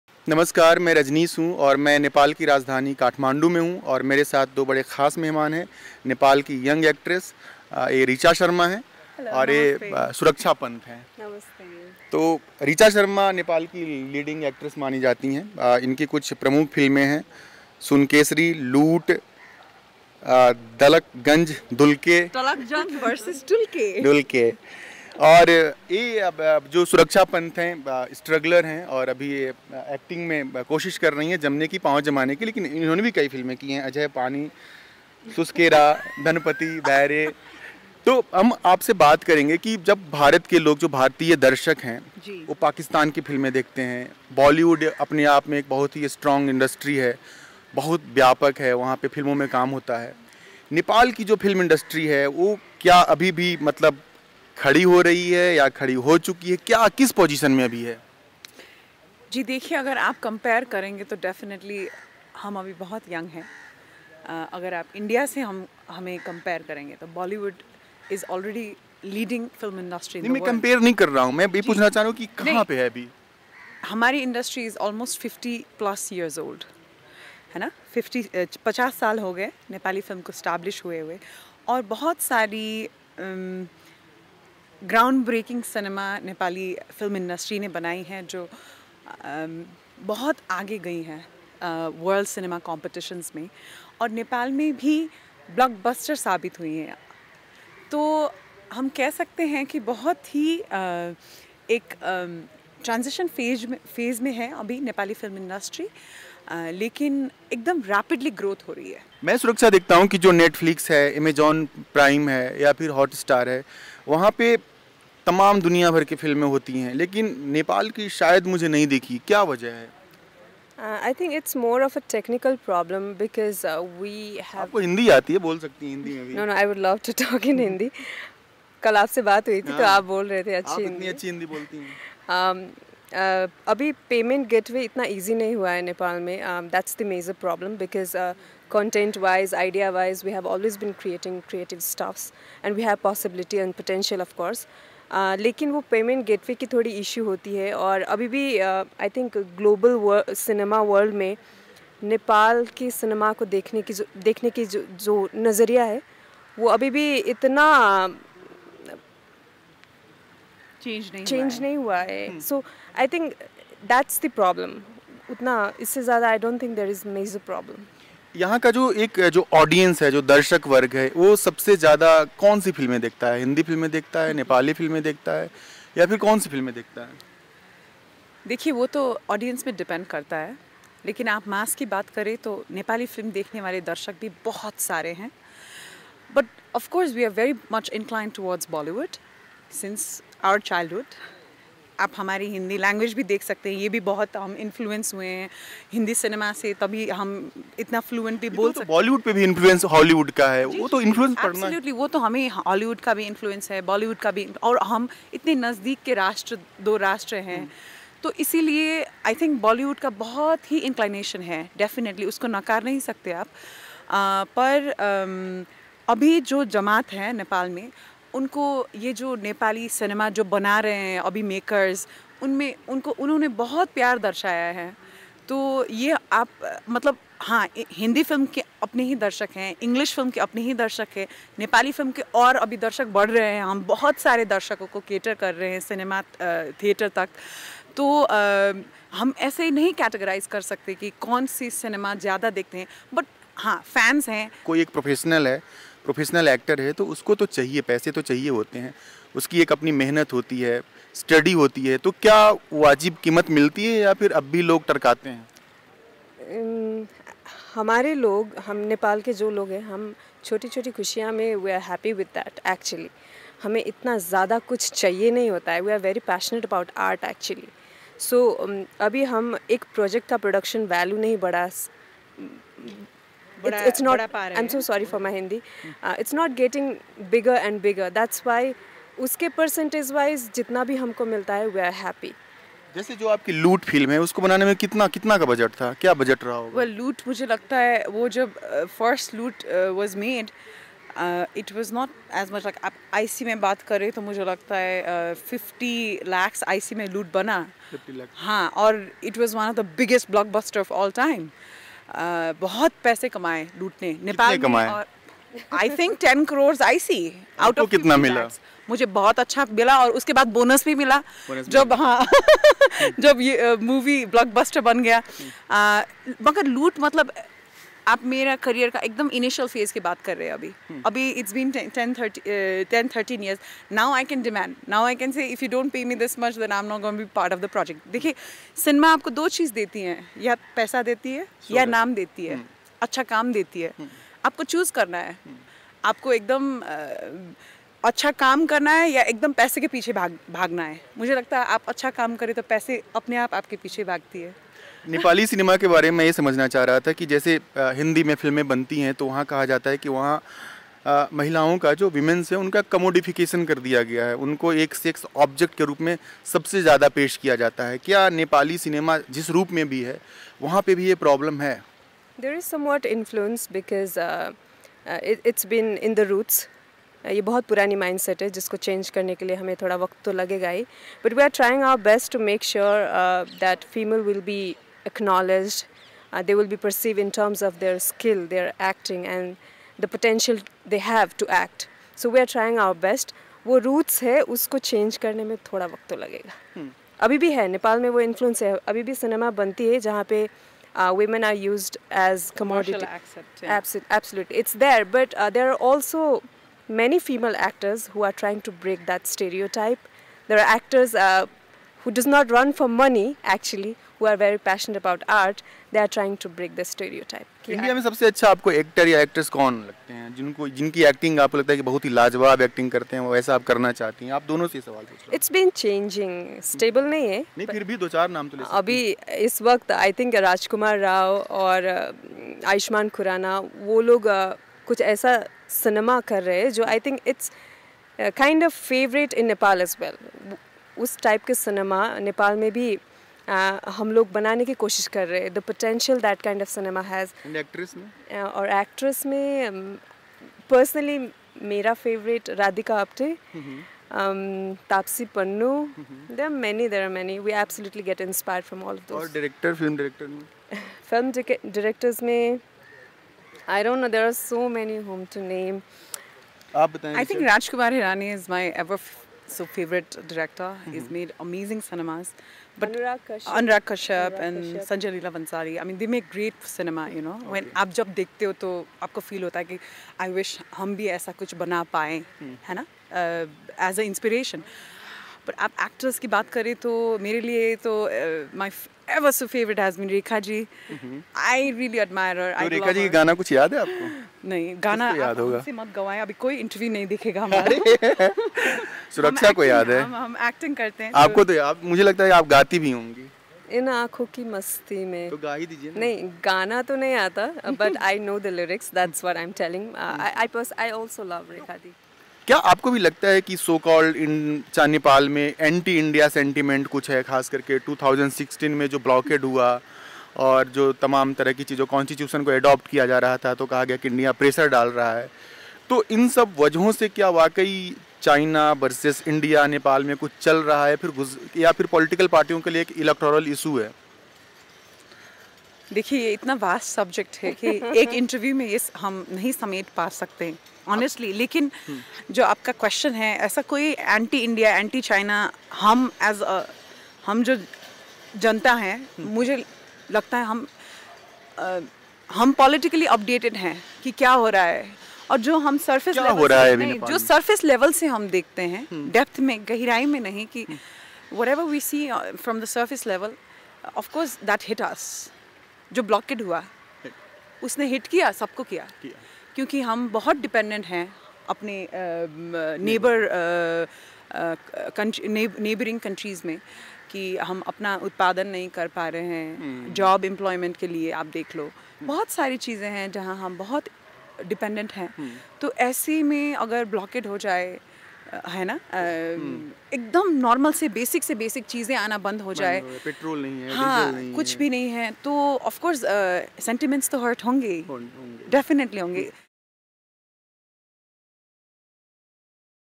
Reecha Sharma and Suraksha Pant : Nepal की दो Actress से ख़ास मुलाक़ात (BBC Hindi)
नेपाल का फिल्म उद्योग भारत की तुलना में काफी छोटा है, लेकिन अपने स्तर पर वहां कोशिशें जारी हैं. बीबीसी टीम हाल में नेपाल में थीं, जहां उसने नेपाली फिल्म उद्योग की दो अभिनेत्रियों से मुलाकात की. ऋचा शर्मा और सुरक्षा पंत, एक वहां की दिग्गज एक्ट्रेस और दूसरी कदम जमाने की कोशिश कर रही हैं. इन दोनों ने भारतीय फिल्म उद्योग, भारत और यहां के सितारों को लेकर क्या-क्या कहा, देखिए इंटरव्यू.

इंटरव्यू: रजनीश कुमार, बीबीसी हिंदी
कैमरा: कमल पेरियार, बीबीसी नेपाली